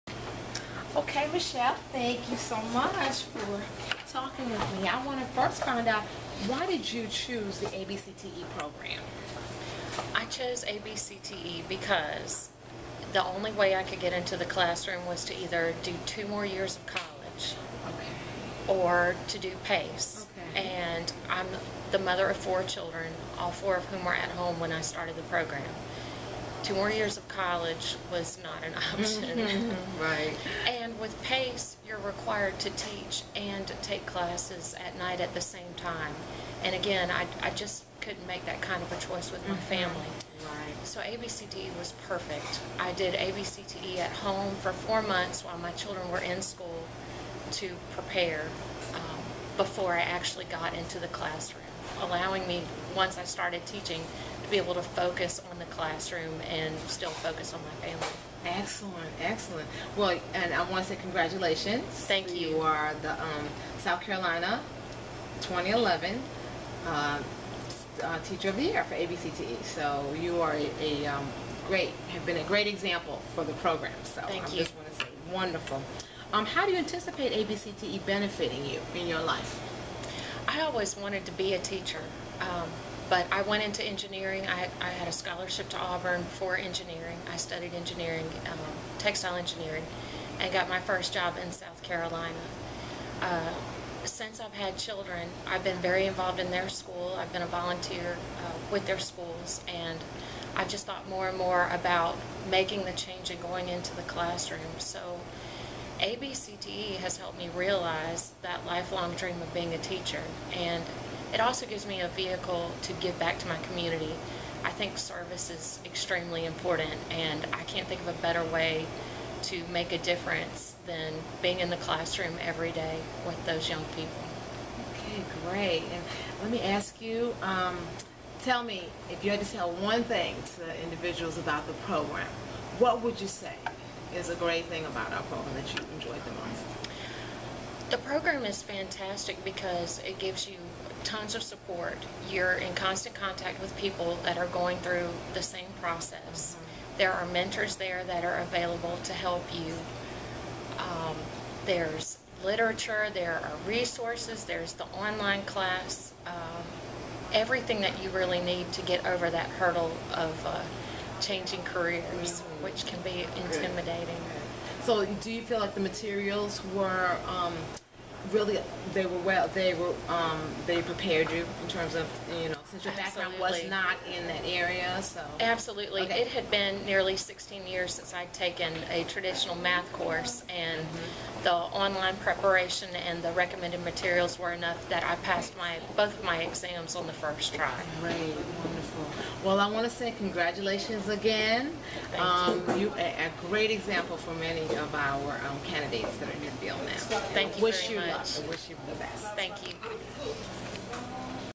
2011 American Board Teacher of the Year Michelle Gilmer
Michelle Gilmer, mathematics teacher at Pendleton High School and American Board Teacher of the Year talks about her pathway to the classroom just before receiving her award at the South Carolina Department of Education, Thursday, Oct. 13 2011